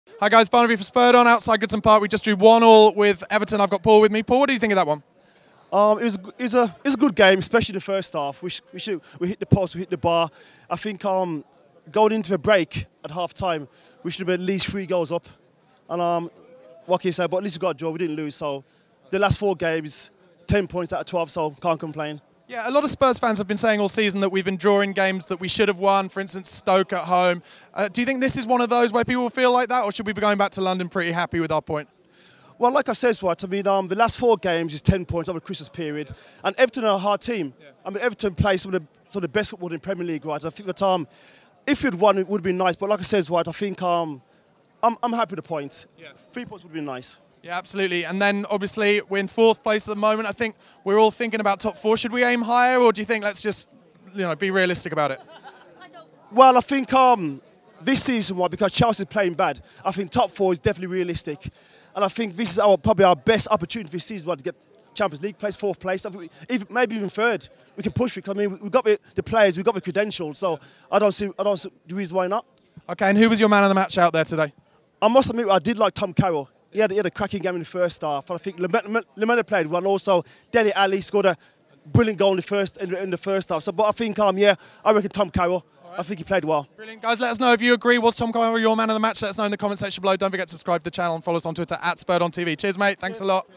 Everton 1-1 Tottenham Hotspur | "Top Four Is Realistic!" | Fan Cam
This Spurs fan feels that getting in the top four is realistic after the Everton 1-1 Spurs game.

Facebook.com/coysnews